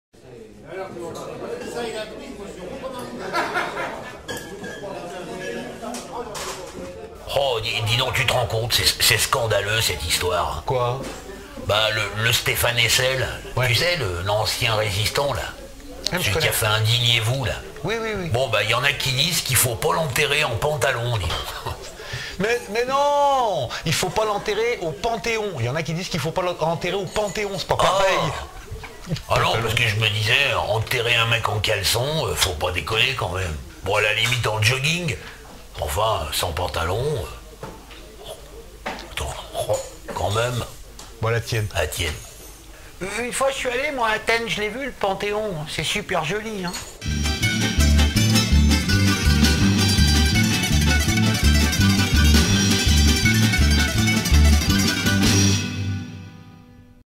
Brève de comptoir- Hommage à Stéphane Hessel
Chacun sa façon de rendre hommage à Stéphane Hessel  (Indignez vous !)
Même dans les bistros on évoque les grands hommes. Bon ! Pas toujours de la façon adéquate mais. le coeur y est quand même ;-)